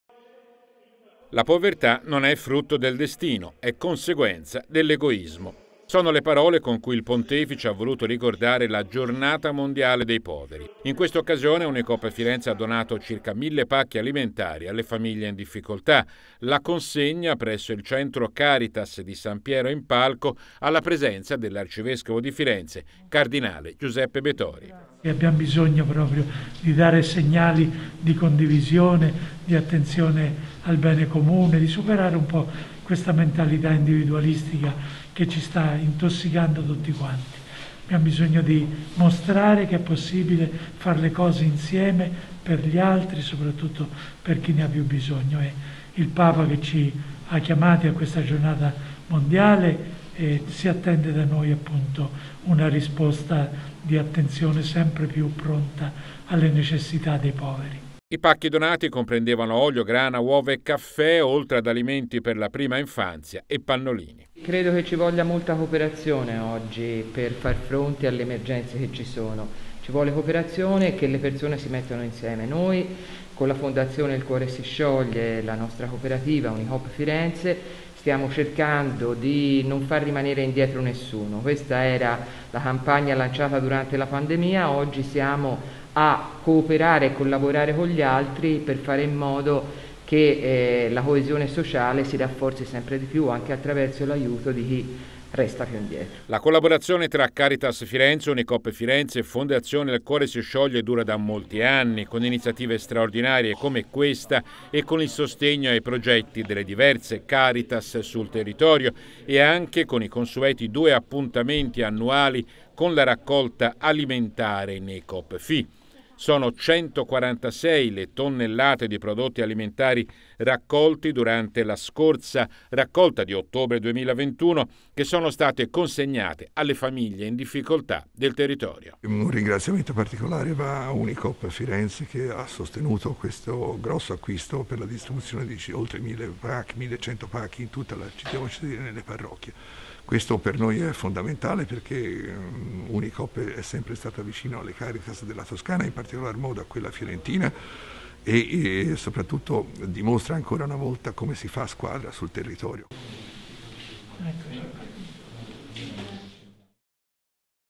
Giornata mondiale dei poveri 2021
In occasione della Giornata mondiale dei poveri, presso la parrocchia di San Piero in Palco, è iniziata la consegna straordinaria di mille pacchi con generi alimentari di prima necessità per le famiglie bisognose di Firenze
( Servizio Informatore Tv, 27-28 novembre 2021)